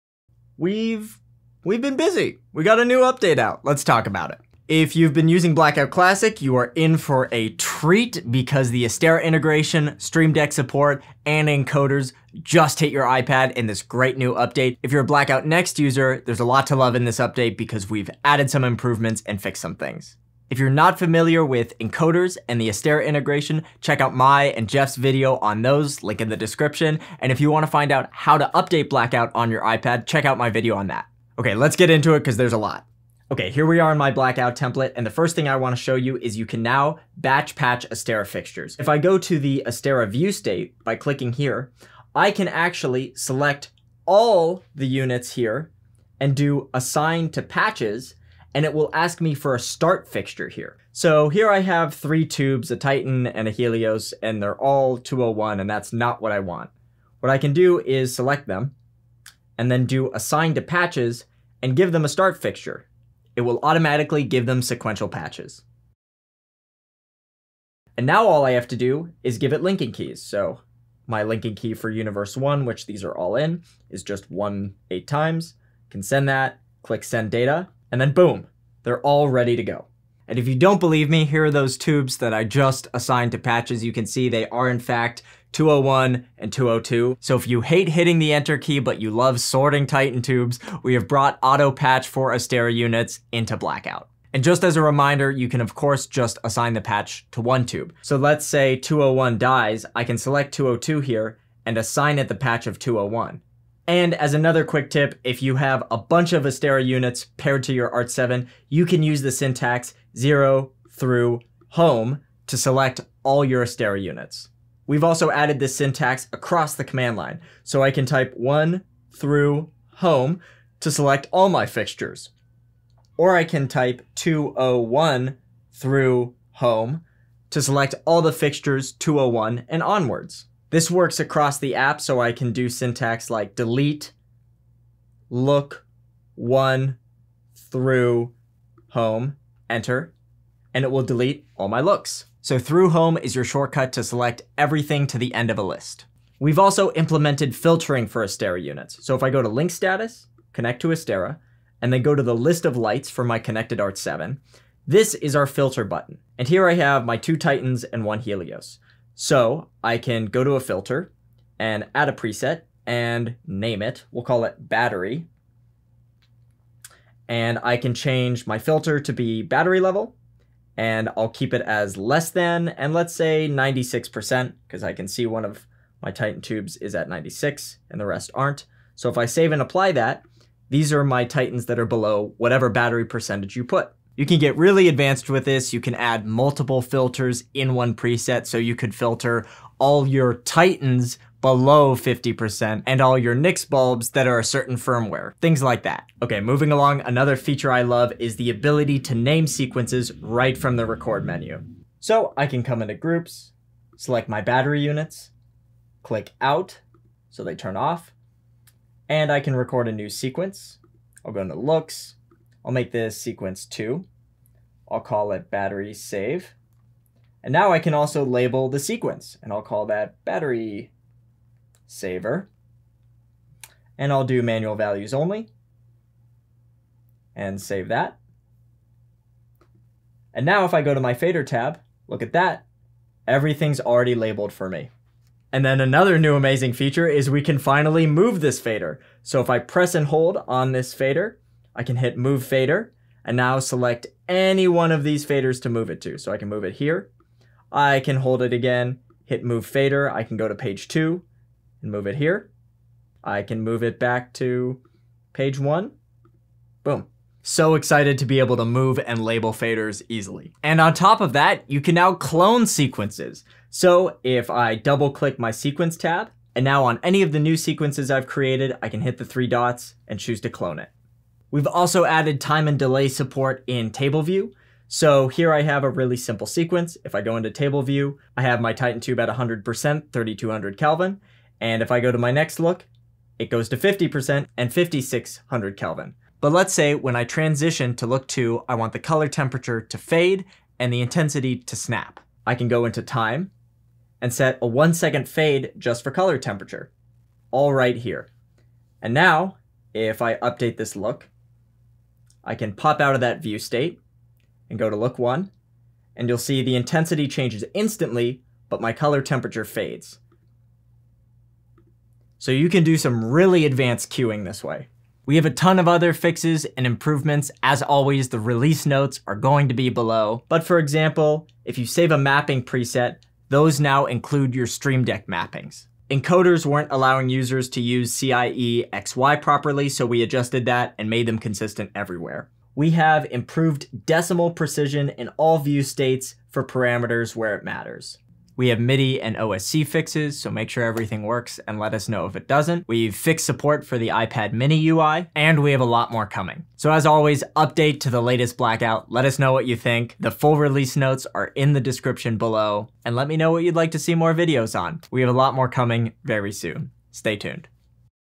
Blackout 2.4.3 Update: Astera Auto Patch, Move Faders, and More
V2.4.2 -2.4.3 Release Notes

✨ NEW FEATURES ✨
 Astera “Assign by Patch” assignment now supports multi-select for auto-configuration!
- In-app purchases replaced with direct website access
- Filter implemented! Filter Astera lights and organize by type
- Free users now indicated in Link Status with alert
- Record Look with optional sequence name
- New command line syntax: “Fix 1 thru Home” will select all Fixtures
- Stream Deck support in MIDI presets
- Improved decimal precision in all view states & encoders
- Flexible CIE x/y handling
- Time/Delay support in Table View
- Sequence cloning is here!

 🛠️ FIXES 🛠️
 - Updated OSC key mapping for fixture control - see docs.blackout-app.com for latest OSC mapping
- Advanced Color Mode update similar to fixture controls (user must select the head of the fader in order to move the fader)
- MIDI fixes
- Fixture control panels now reset correctly when no fixtures are selected
- Improved battery info retrieval for Astera fixtures
- Blocked saving of empty Favorites
- All faders now save properly when deleted from command ‘Delete Fader 1 thru x`
- Unnumbered Astera fixtures now act as fixture 0 (command example “Fix 0” will select all)
- Show 2–20h range in DMX Configurator
- Export and cloning custom views
- CRMX signal now shows 0–100% instead of 0–255
- iPad mini UI fixes
- OSC now shows up in MIDI Learn
- Smoother encoder UI
- Fixed infinite popup from hardware
- Better DMX patching & pan/tilt MIDI
- UI polish and improved fader handling
- More stable fixture and sidebar behavior
- Bug fixes and performance enhancements